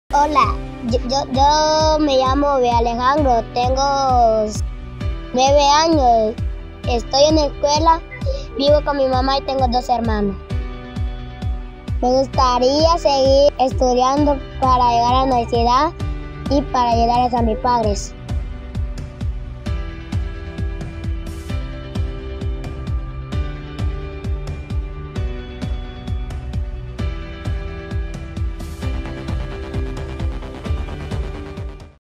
Cambiando vidas.
Obed Alejandro, es uno de los niños que estan en el proyecto "EDUCAME," como este hay muchos casos más a los que podemos alcanzar por medio de tu apoyo.